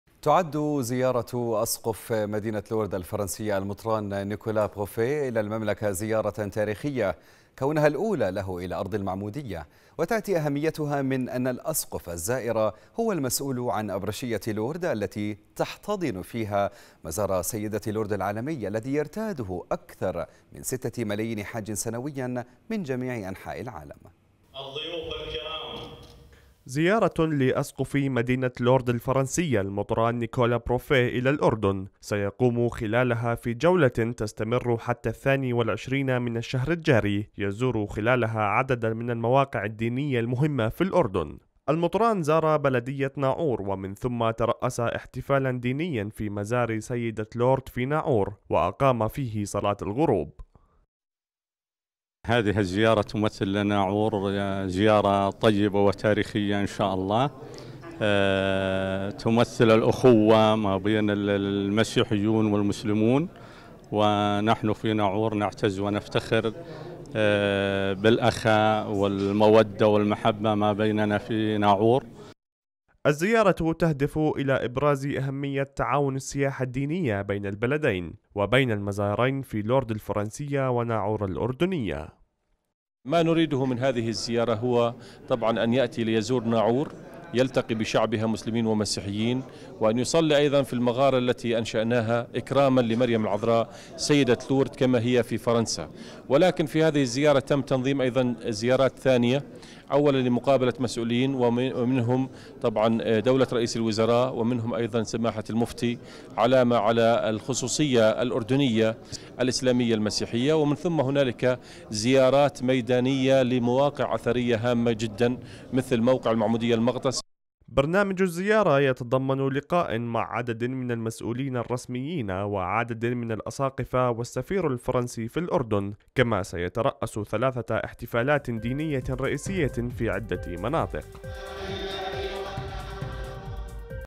أسقف مدينة لورد الفرنسية يزور الأردن ويشارك في عدة احتفالات دينية - (19-9-2018)
تعّد زيارة أسقف مدينة لورد الفرنسية المطران نيكولا بروفيه إلى المملكة تاريخية كونها الأولى له إلى أرض المعمودية وتأتي أهميتها لأن الأسقف الزائر هو المسؤول عن أبرشية لورد التي تحتضن فيها مزار سيدة لورد العالمي الذي يؤمّه أكثر من ستة ملايين حاج سنويًا من جميع أنحاء العالم
تقرير: عمر فرام